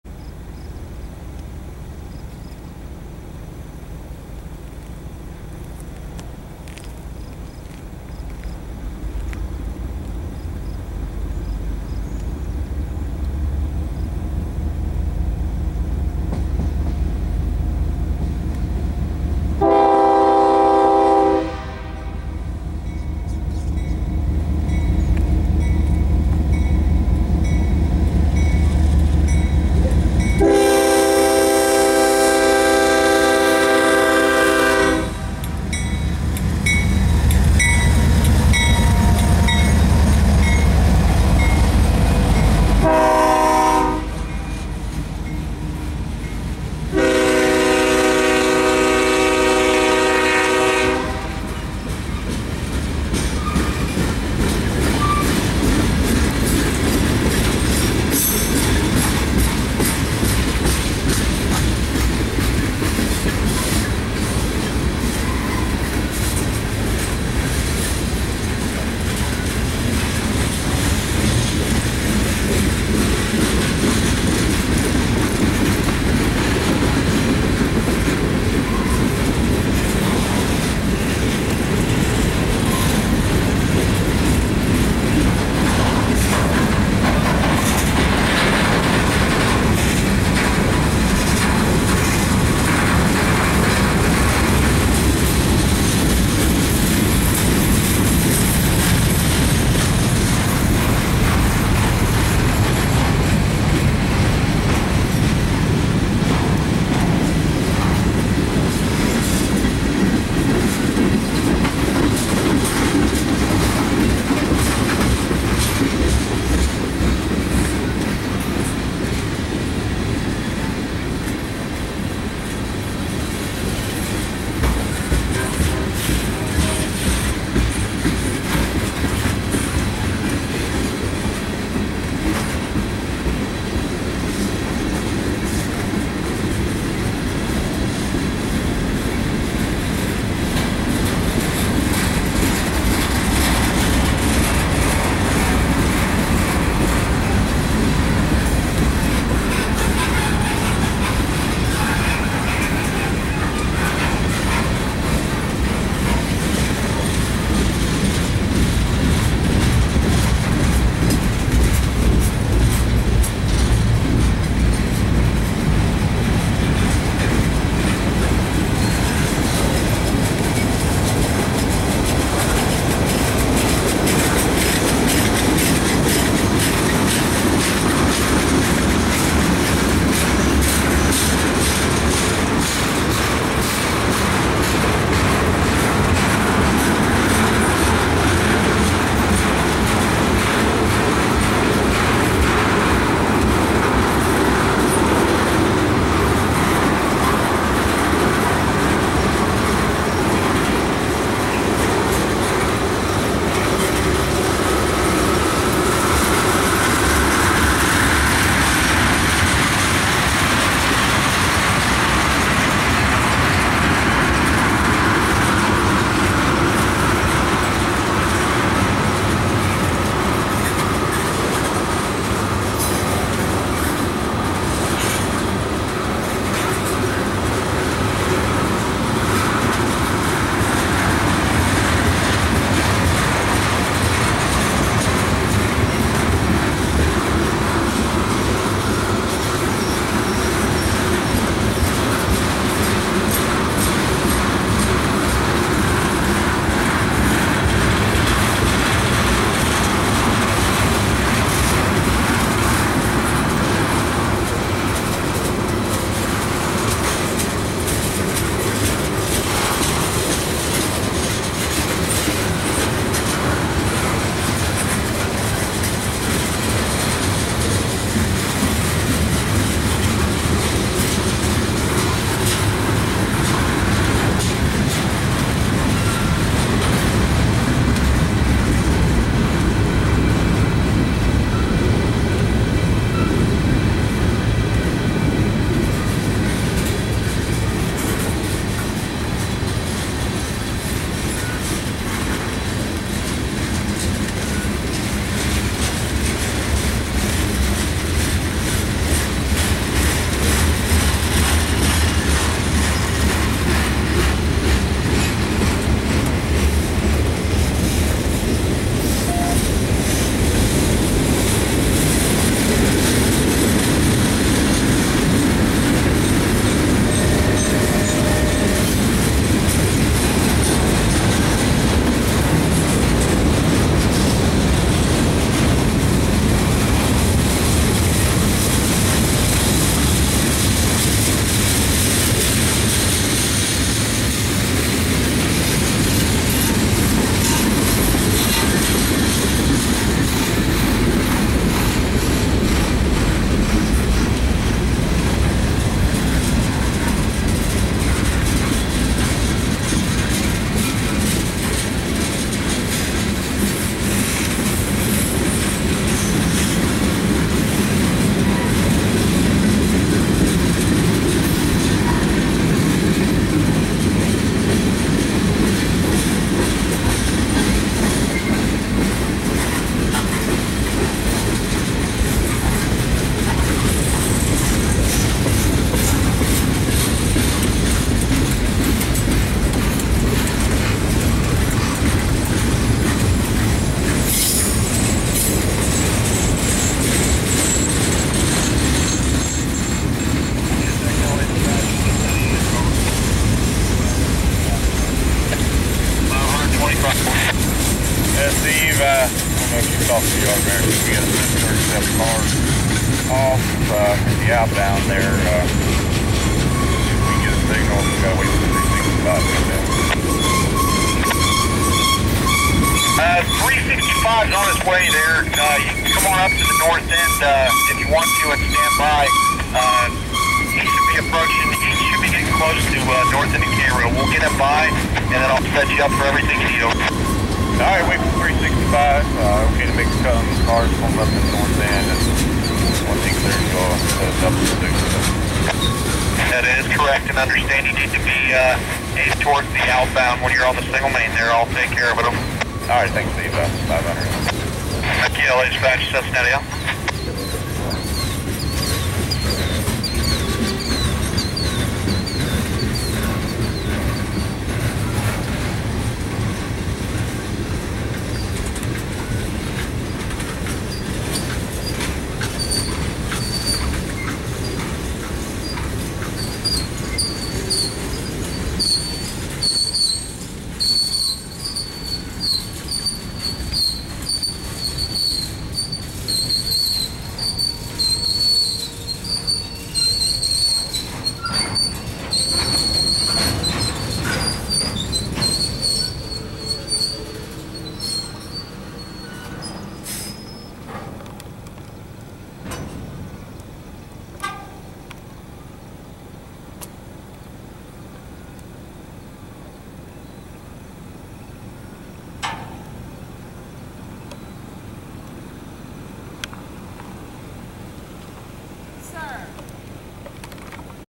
Massive CSX mixed freight in Lima Ohio!
A northbound on the Toledo Sub coming into the yard in Lima. 6-25-15